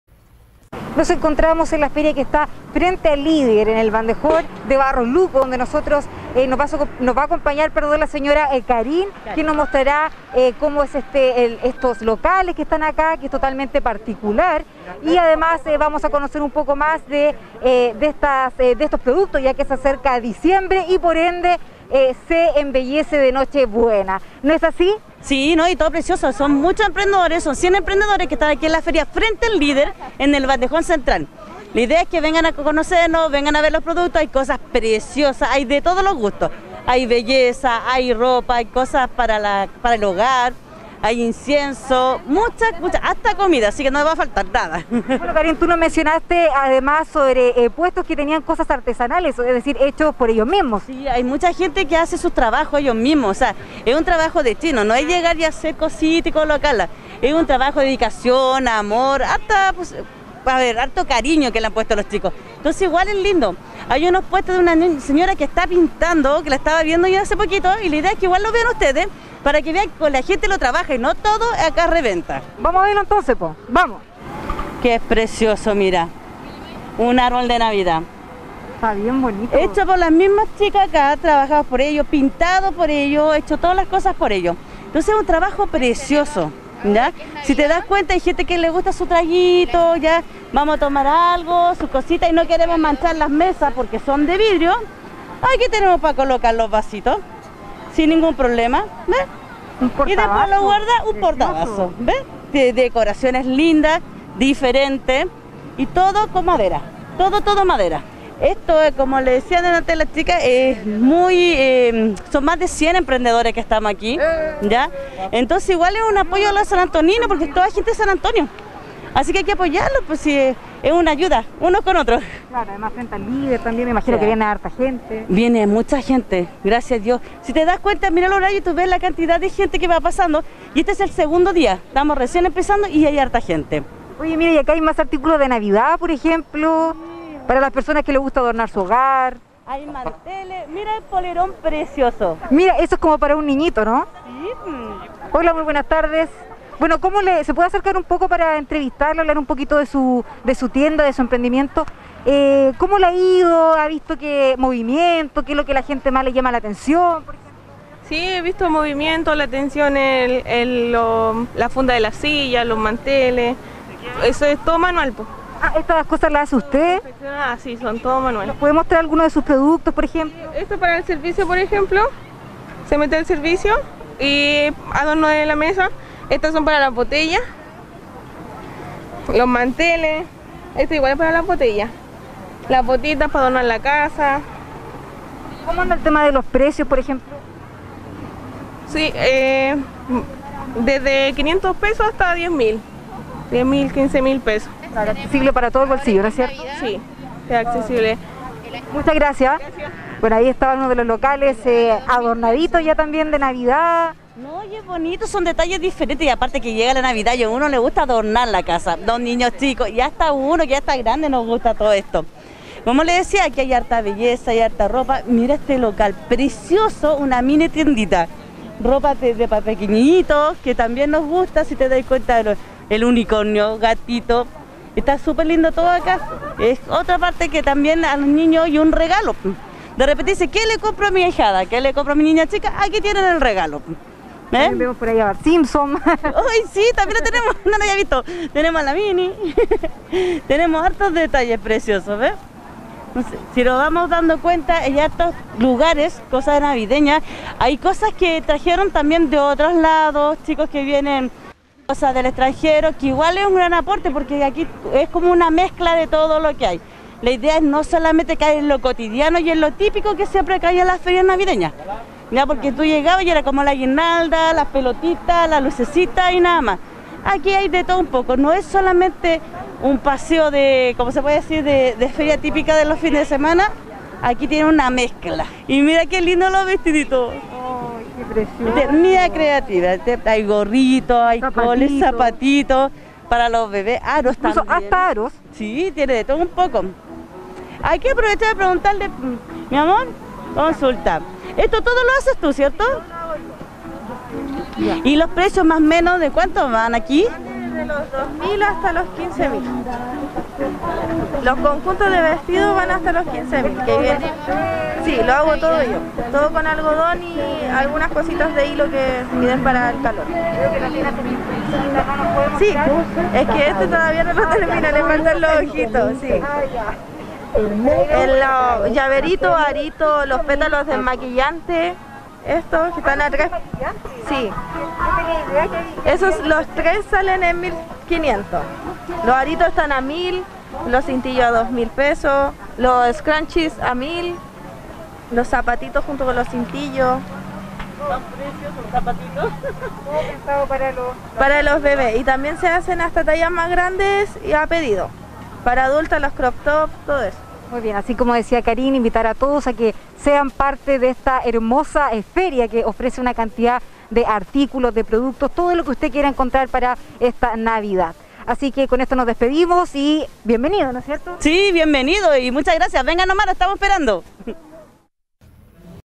02 DIC 2020 LA EXPO NAVIDAD ABRIÓ SUS PUERTAS EN AVENIDA BARROS LUCO
LA FERIA EXPO NAVIDAD SE INSTALÓ EN EL BANDEJÓN CENTRAL DE AVENIDA BARROS LUCO, TODA UNA OPORTUNIDAD PARA AYUDAR AL VIEJITO PASCUERO EN ESTA NAVIDAD, YA QUE CUENTA CON 100 PUESTOS DE EMPRENDEDORES LOCALES. PARA SABER MÁS SOBRE ESTE TEMA TOMAMOS CONTACTO CON NUESTRA COLEGA CATALINA OTAIZA. QUIEN NOS ENTREGA MÁS ANTECEDENTES  A CONTINUACIÓN